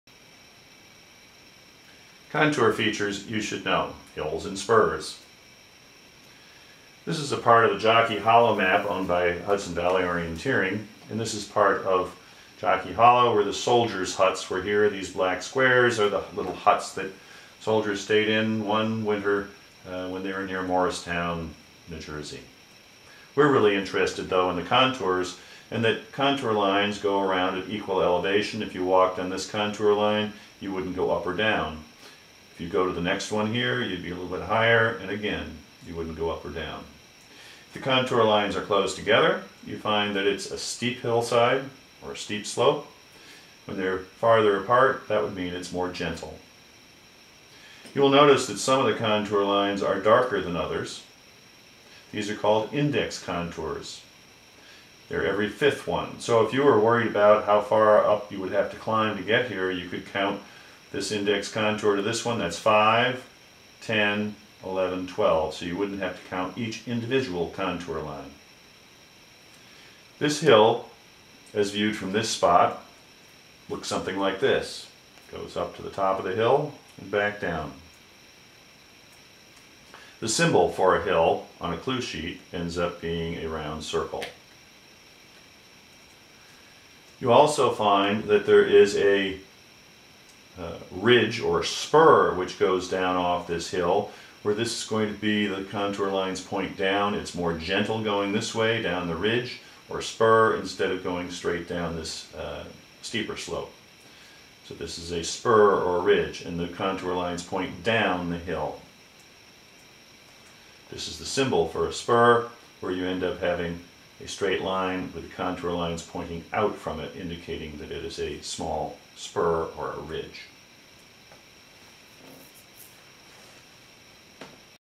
Orienteering Hills and Spurs
Orienteering lesson on how to understand contours showing hills and spurs